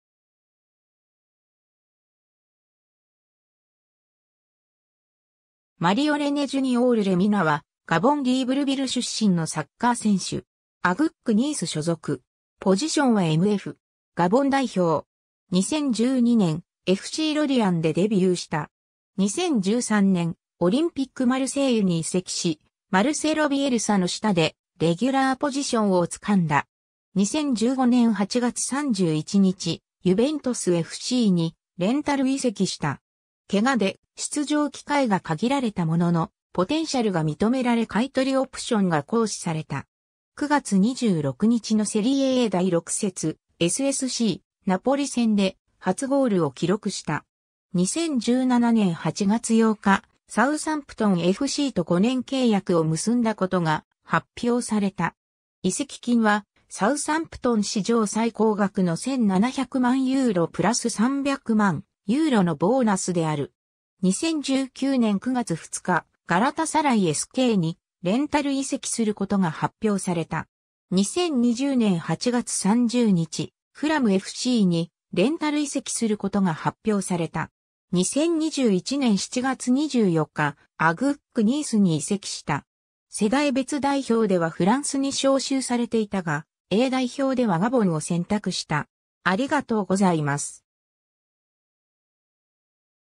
マリオ・レミナ
マリオ・レネ・ジュニオール・レミナ（Mario René Junior Lemina, 1993年9月1日 - ）は、ガボン・リーブルヴィル出身のサッカー選手。
OGCニース所属。
ポジションはMF。
ガボン代表。
 2012年、FCロリアンでデビューした。
 2013年、オリンピック・マルセイユに移籍し、マルセロ・ビエルサの下でレギュラーポジションをつかんだ。
 2015年8月31日、ユヴェントスFCにレンタル移籍した。
怪我で出場機会が限られたものの、ポテンシャルが認められ買い取りオプションが行使された。
9月26日のセリエA第6節、SSCナポリ戦で初ゴールを記録した。
 2017年8月8日、サウサンプトンFCと5年契約を結んだことが発表された。
移籍金はサウサンプトン史上最高額の1700万ユーロ+300万ユーロのボーナスである。
 2019年9月2日、ガラタサライSKにレンタル移籍することが発表された。
 2020年8月30日、フラムFCにレンタル移籍することが発表された。
 2021年7月24日、OGCニースに移籍した。
 世代別代表ではフランスに召集されていたが、A代表ではガボンを選択した。